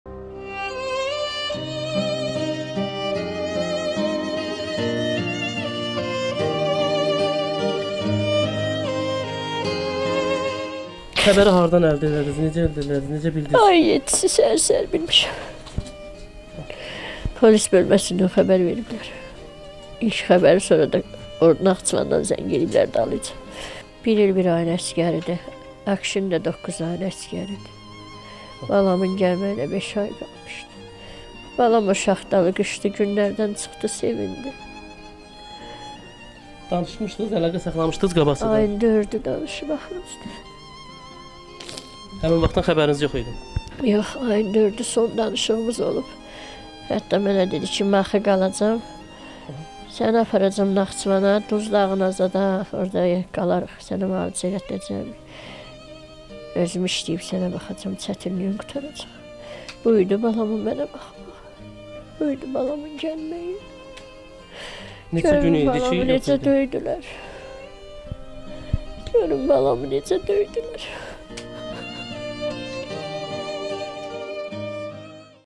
Ermənilərin ağır işgəncə verdiyi Hüseyn Axundovun anası açıqlama verdi
Məlumat verdiyimiz kimi, bir neçə gün əvvəl Naxçıvan Muxtar Respublikasının Şahbuz rayonunun Ermənistanla sərhədboyu ərazisində əlverişsiz hava şəraitində məhdud görmə səbəbindən Azərbaycan ordusunun itkin düşmüş əsgərləri - 2004-cü il təvəllüdlü Bəbirov Aqşin Qabil oğlu və 2003-cü il təvəllüdlü Axundov Hüseyn Əhliman oğlu Ermənistan tərəfindən əsir götürülüb.
 Xezerxeber.az-ın məlumatına görə, əsir götürülmüş Axundov Hüseyn Əhliman oğluna qarşı ermənilər tərəfindən ağır fiziki işgəncə və qeyri-insani rəftar halları olub.

Hüseyn Axundovun anası Şəlalə Axundova "Xəzər Xəbər"ə açıqlamasında hadisə ilə bağlı danşıb, oğlu ilə bağlı fikirlərini bildirib.